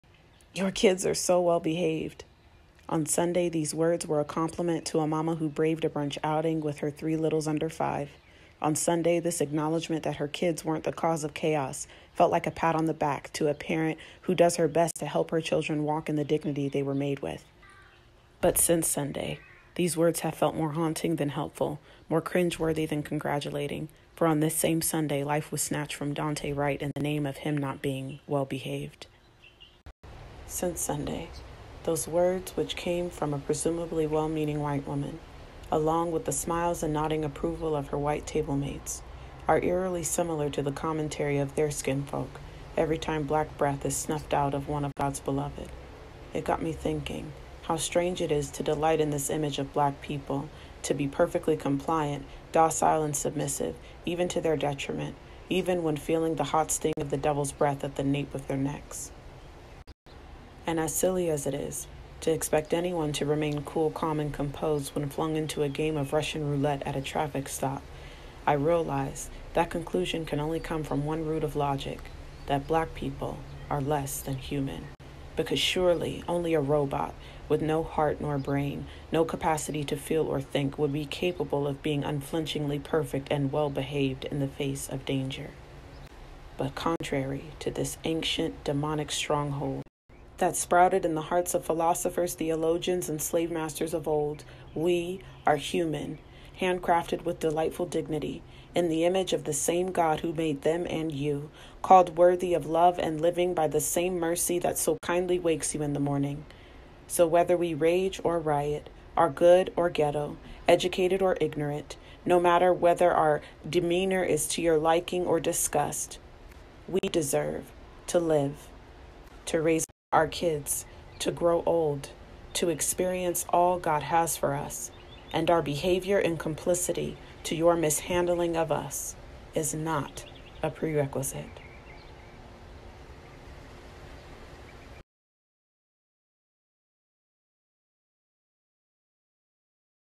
Well Behaved | A Poem Honoring
A poem I wrote in the height of 2021's violence in America to honor Daunte Wright and the lives of Black Americans that have been lost needlessly to the evils of racism, police brutality and gun violence.